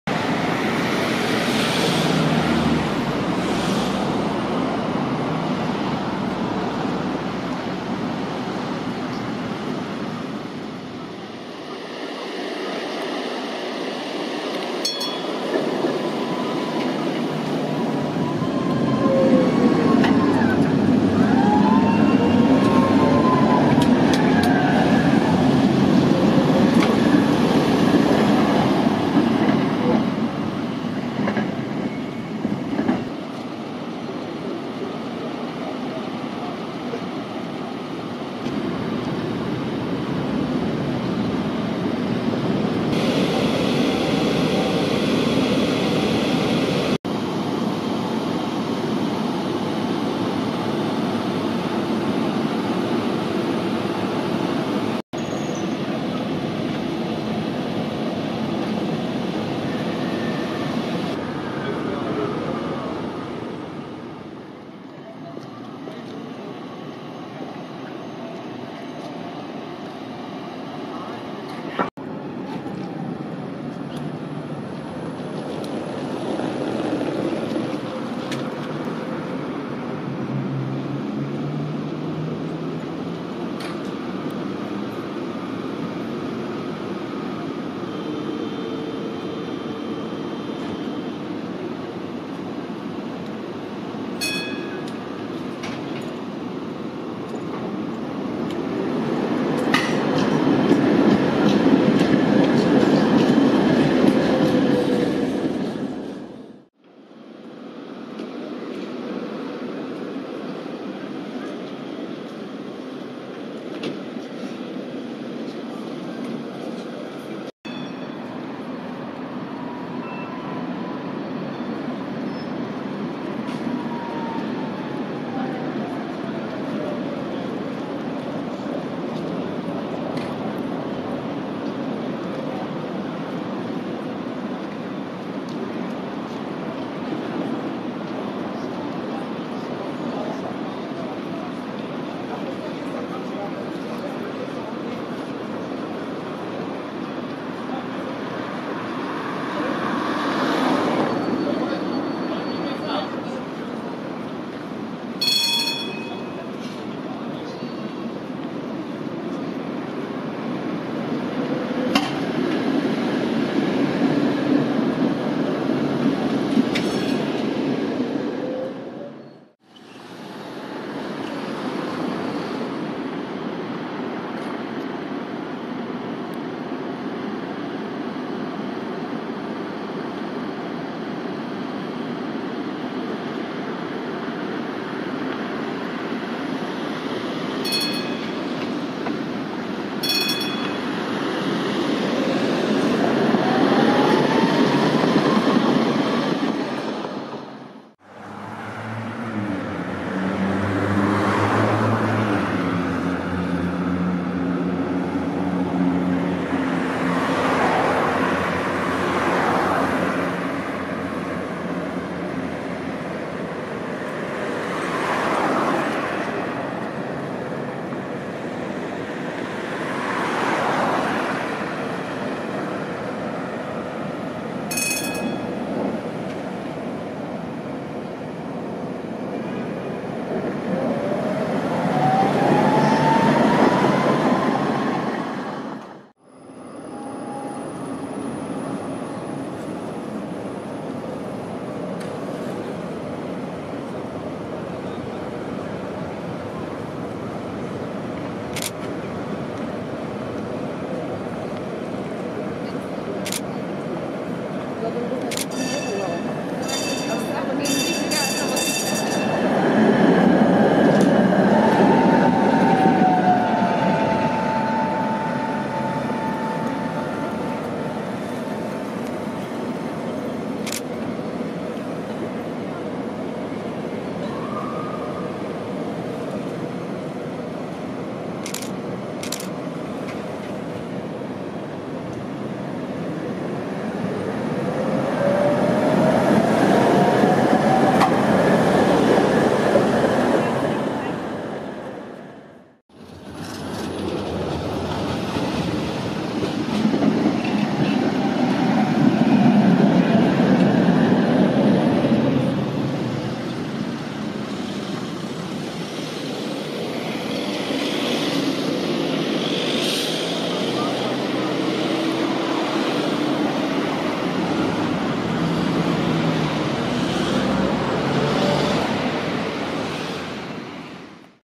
Linka 24 - poslední den jízdy pražských tramvají T3SUCS v běžném provozu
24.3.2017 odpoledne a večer